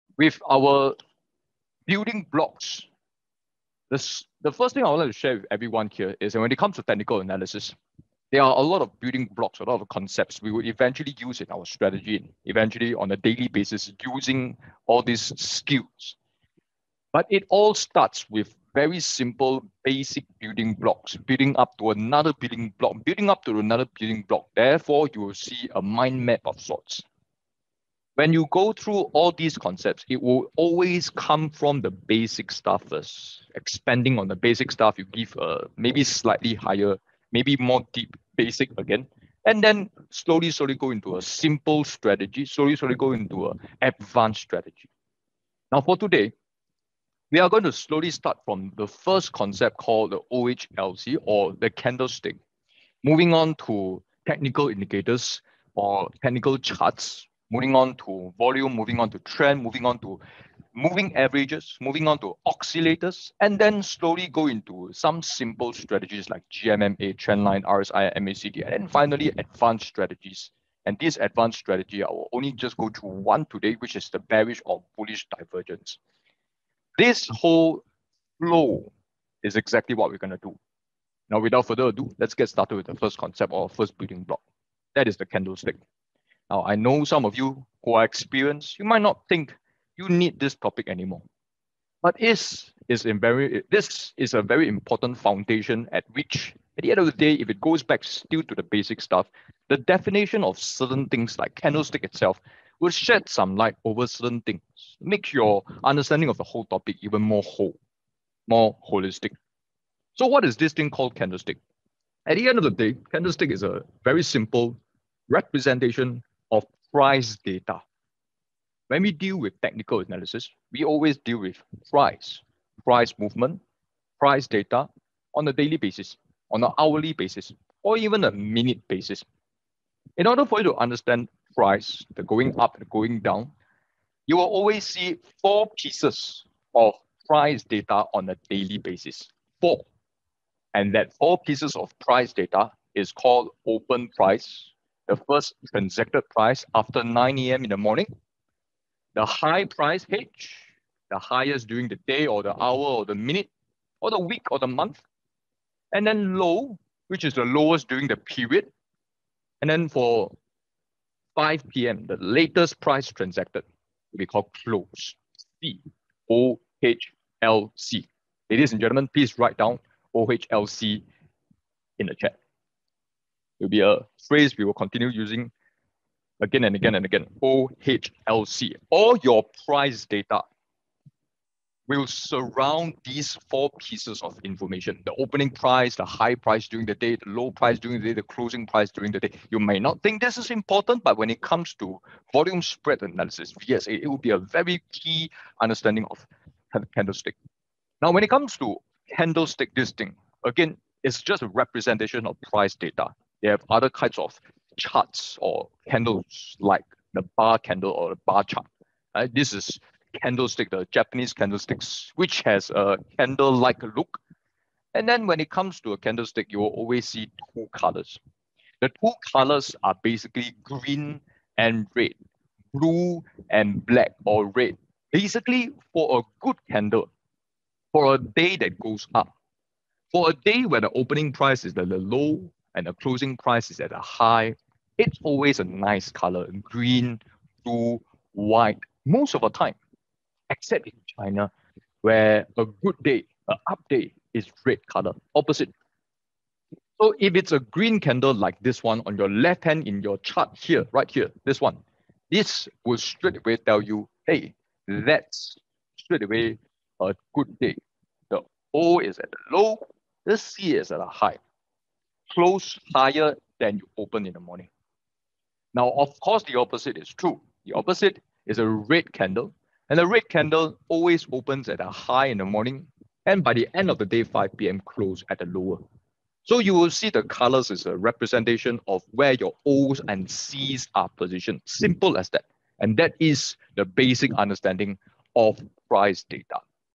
Candlestick OHLC
When it comes to technical analysis, there are a lot of building blocks and concepts that we use in our strategy.

Here's a short clip of what I covered in my training on OHLC Candlesticks with my students.

If you enjoy how I explain and teach my students,